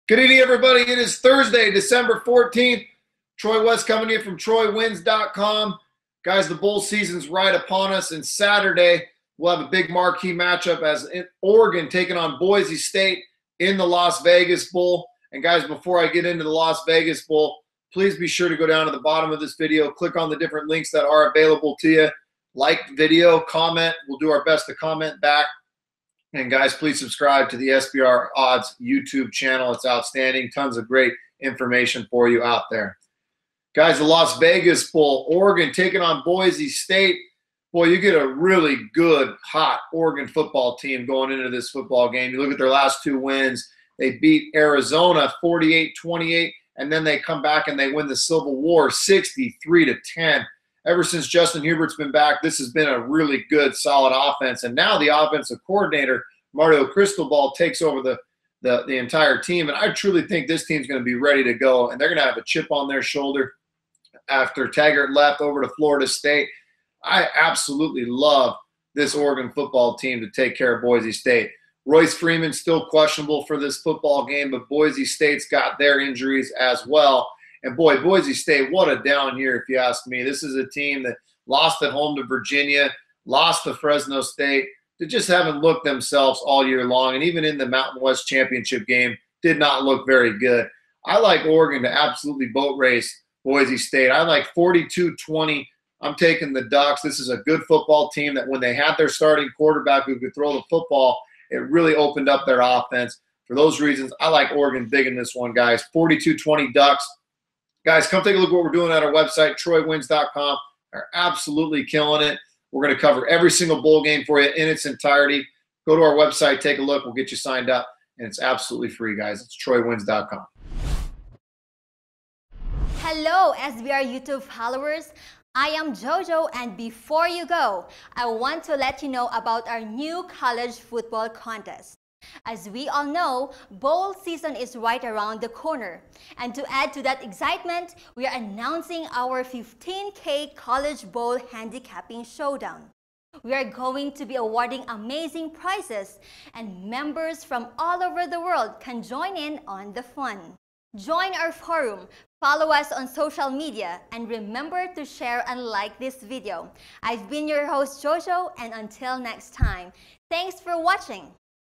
Las Vegas Bowl Free Pick | Oregon vs Boise State | Betting Preview & Analysis
Oregon and Boise State battle in the Las Vegas Bowl this coming Saturday. Oregon has there QB back and has been playing outstanding football ever since. I expect the bigger, faster, stronger team to win this one big. 42-20 DUCKS.